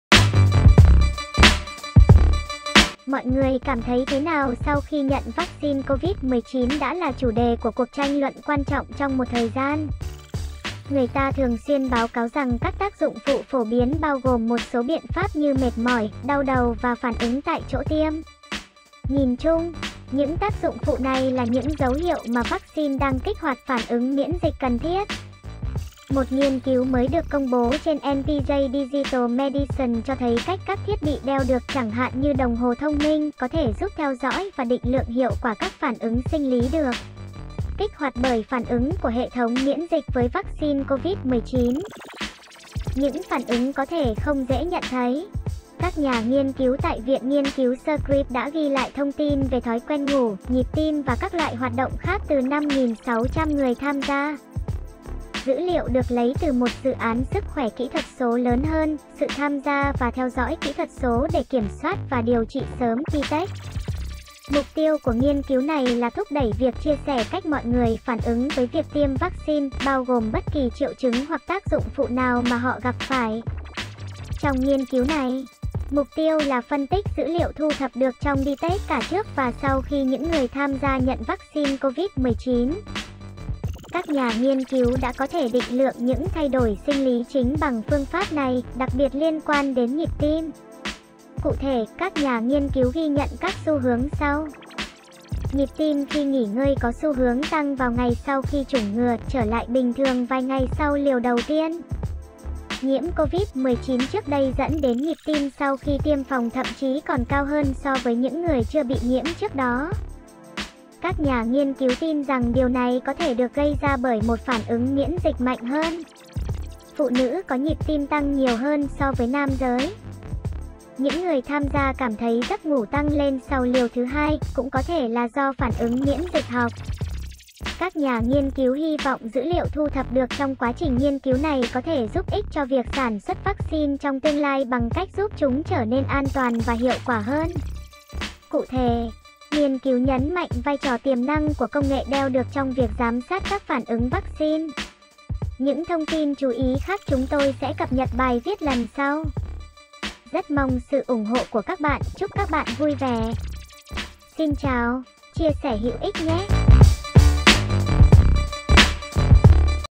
Thiết bị đeo tay Làm nổi bật các phản ứng sinh lý đối với vắc xin COVID 19
Mọi người cảm thấy thế nào sau khi nhận vắc-xin COVID-19 đã là chủ đề của cuộc tranh luận quan trọng trong một thời gian. Người ta thường xuyên báo cáo rằng các tác dụng phụ phổ biến bao gồm một số biện pháp như mệt mỏi, đau đầu và phản ứng tại chỗ tiêm. Nhìn chung, những tác dụng phụ này là những dấu hiệu mà vắc xin đang kích hoạt phản ứng miễn dịch cần thiết.

Một nghiên cứu mới được công bố trên npj Digital Medicine cho thấy cách các thiết bị đeo được (chẳng hạn như đồng hồ thông minh) có thể giúp theo dõi và định lượng hiệu quả các phản ứng sinh lý được kích hoạt bởi phản ứng của hệ thống miễn dịch với vắc xin COVID-19, những phản ứng có thể không dễ nhận thấy.

Các nhà nghiên cứu tại Viện nghiên cứu Scripps đã ghi lại thông tin về thói quen ngủ, nhịp tim và các loại hoạt động khác từ 5.600 người tham gia. Dữ liệu được lấy từ một dự án sức khỏe kỹ thuật số lớn hơn, Sự tham gia và Theo dõi Kỹ thuật số để Kiểm soát và Điều trị Sớm (DETECT) . Mục tiêu của nghiên cứu này là thúc đẩy việc chia sẻ cách mọi người phản ứng với việc tiêm vắc-xin, bao gồm bất kỳ triệu chứng hoặc tác dụng phụ nào mà họ gặp phải.

Trong nghiên cứu này, mục tiêu là phân tích dữ liệu thu thập được trong DETECT cả trước và sau khi những người tham gia nhận vắc xin COVID-19. Các nhà nghiên cứu đã có thể định lượng những thay đổi sinh lý chính bằng phương pháp này, đặc biệt liên quan đến nhịp tim. Cụ thể, các nhà nghiên cứu ghi nhận các xu hướng sau:

Nhịp tim khi nghỉ ngơi có xu hướng tăng vào ngày sau khi chủng ngừa, trở lại bình thường vài ngày sau liều đầu tiên.

Nhiễm COVID-19 trước đây dẫn đến nhịp tim sau khi tiêm phòng thậm chí còn cao hơn so với những người chưa bị nhiễm trước đó. Các nhà nghiên cứu tin rằng điều này có thể được gây ra bởi một phản ứng miễn dịch mạnh hơn.

Phụ nữ có nhịp tim tăng nhiều hơn so với nam giới.

Những người tham gia cảm thấy giấc ngủ tăng lên sau liều thứ hai, cũng có thể là do phản ứng miễn dịch học.

Các nhà nghiên cứu hy vọng dữ liệu thu thập được trong quá trình nghiên cứu này có thể giúp ích cho việc sản xuất vắc xin trong tương lai bằng cách giúp chúng trở nên an toàn và hiệu quả hơn. Cụ thể, nghiên cứu nhấn mạnh vai trò tiềm năng của công nghệ đeo được trong việc giám sát các phản ứng vắc xin.